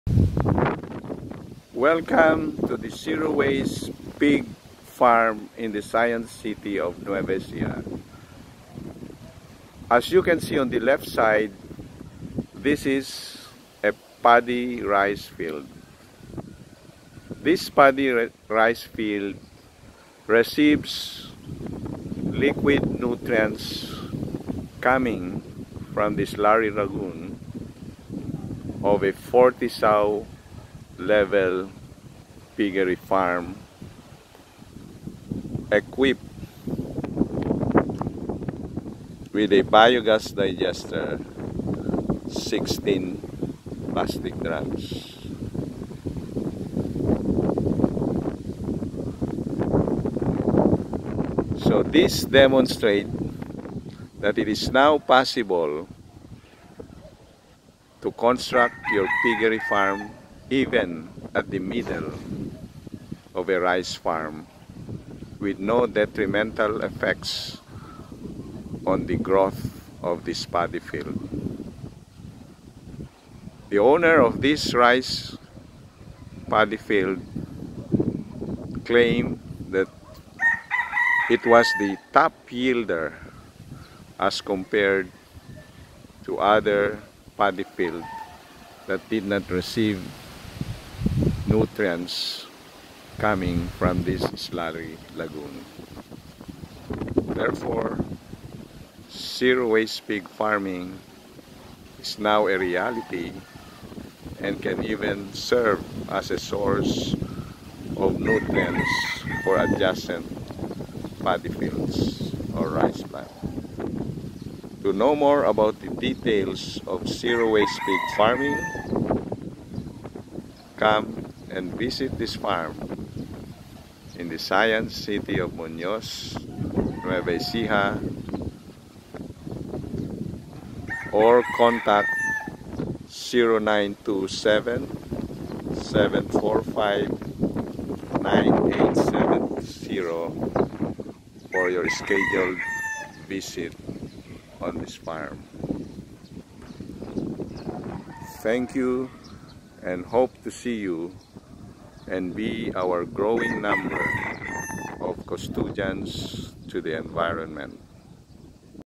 Pig Slurry Lagoon as a Liquid Fertilizer for Palay Farm in the Philippines
This slurry lagoon was applied with a Bioplus Activator to make it possible as an organic fertilizer.